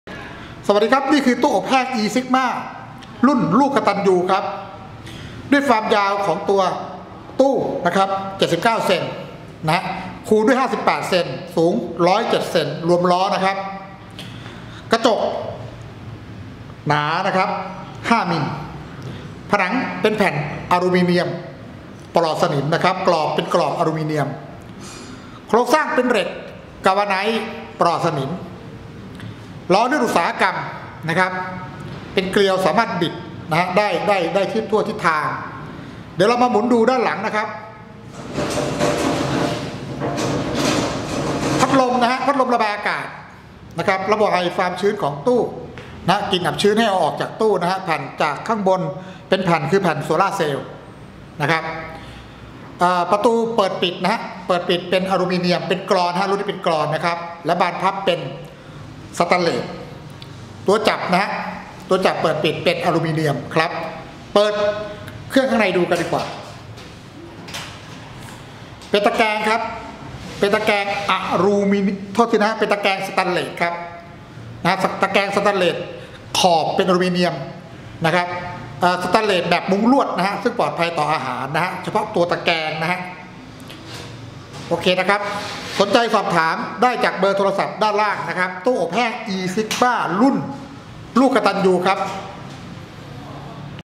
ตู้อบแห้ง ESIGMA พลังงานแสงอาทิตย์ แบบเรือนกระจก
ตากกล้วยน้ำว้าได้ครั้งละ 100 ลูก  อบแห้งเร็วกว่า ประหยัดกว่า สะอาดกว่า
เหมาะสำหรับ มีไว้ประจำบ้าน เพื่อใช้ในการถนอมอาหาร อบแห้ง เช่น เนื้อ ปลา พืช ผัก ผลไม้ เครื่องเทศ พริก ใบมะกรูด กล้วย ฯลฯ

ราคาและรายละเอียดเพิ่มเติม